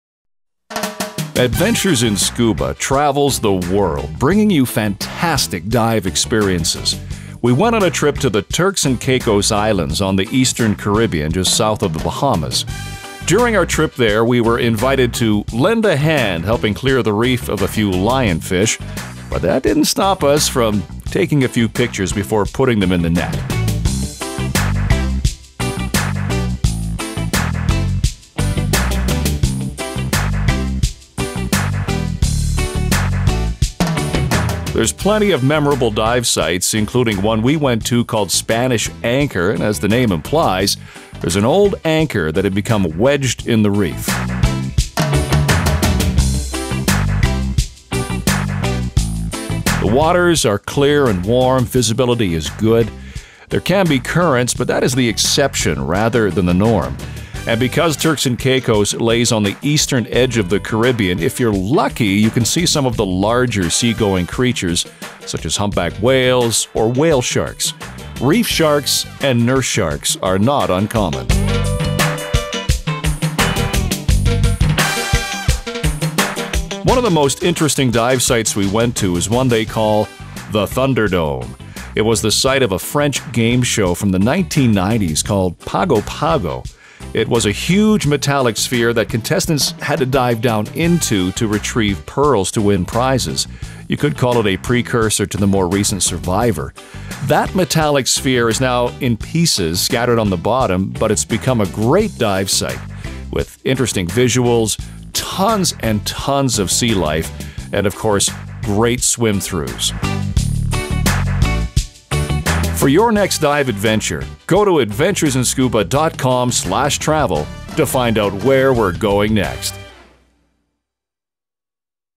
Adventures in Scuba Turks & Caicos Trip Video
The Turks & Caicos Islands are another great dive destination that Adventures in Scuba takes you to. We helped hunt lionfish - a menace to all reefs in the Caribbean. We saw more than a few sharks, barracudas plus an old anchor wedged into the reef and an old French game show dive sight they now call teh Thunderdome.